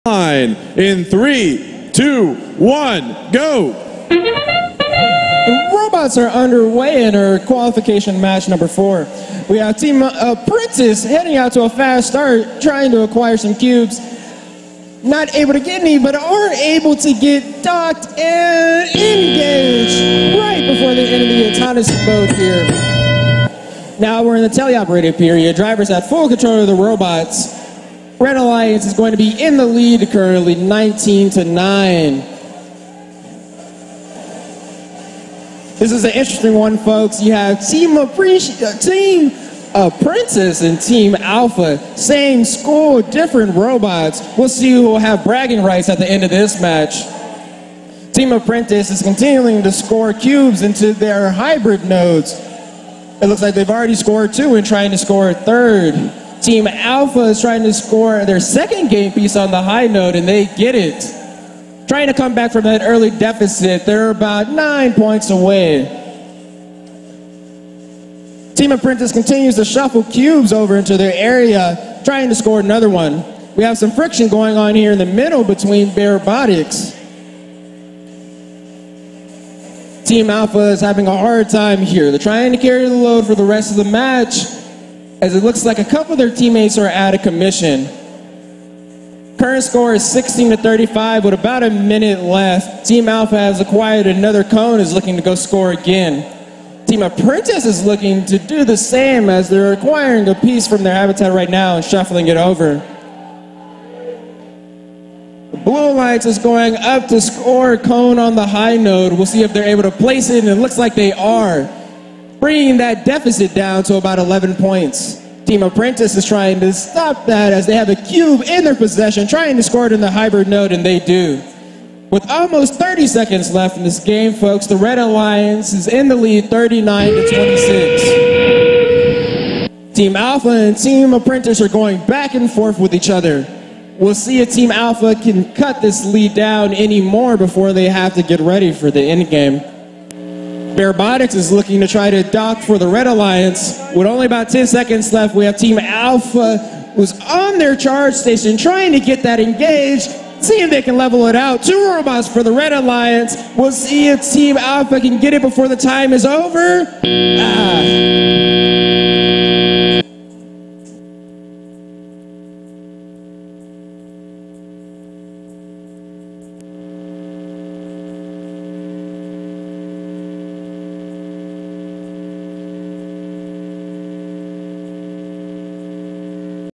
Dallas 2023 Q4
Qualification Match - Dallas District Event 2023
Red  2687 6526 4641 Score: 68
Blue: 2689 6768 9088 Score: 37
FIRST Robotics Competition
Date: 3/4/2023